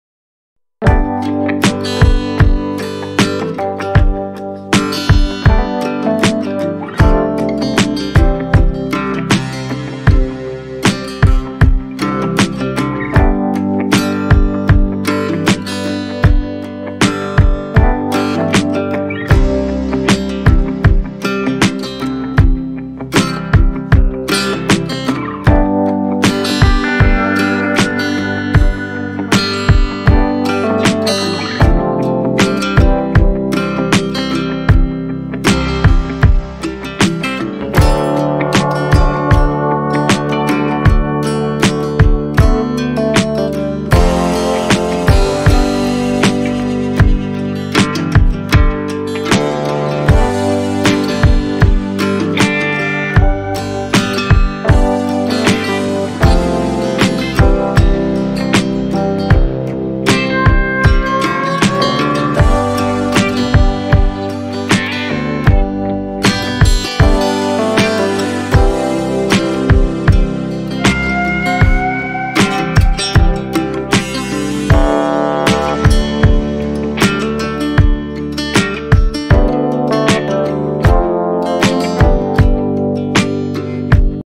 What are the two strange things the guru and his disciple find in the Kingdom of Fools ? | Class 9
In this video you will learn the answer of the Q. What are the two strange things the guru and his disciple find in the Kingdom of Fools? from the chapter 'In the Kingdom of Fools'. Class 9 English Supplimentary. Chapter 4